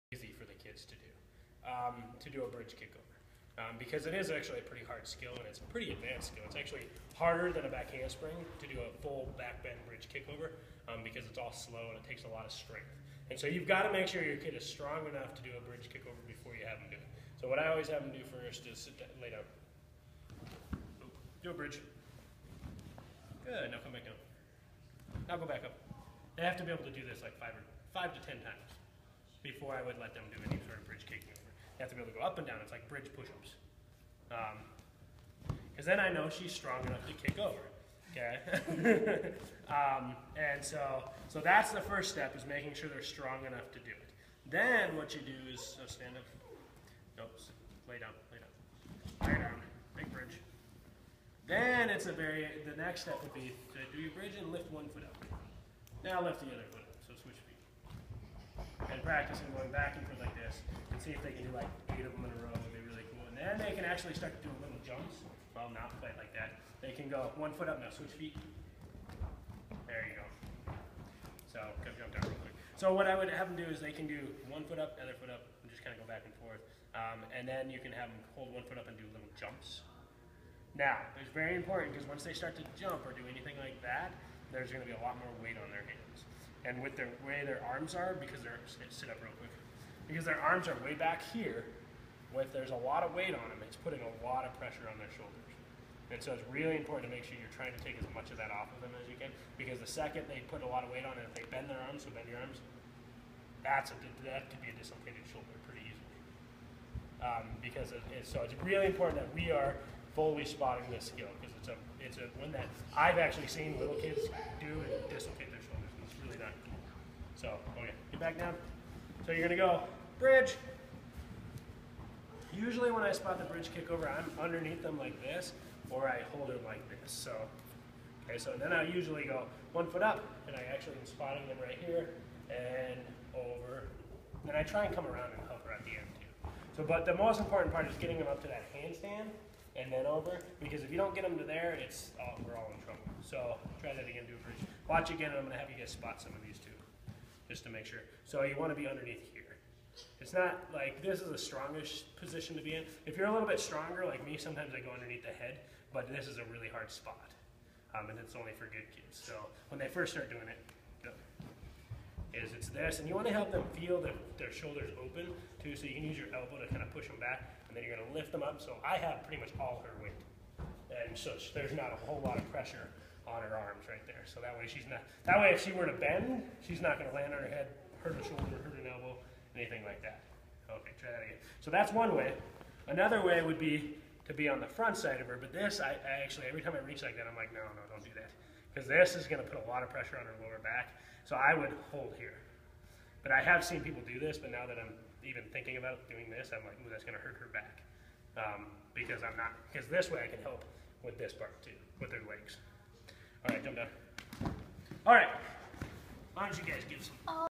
Spotting Floor: Bridge Kickover, Part 1
Chris explains drills for starting to train the bridge kickover on floor.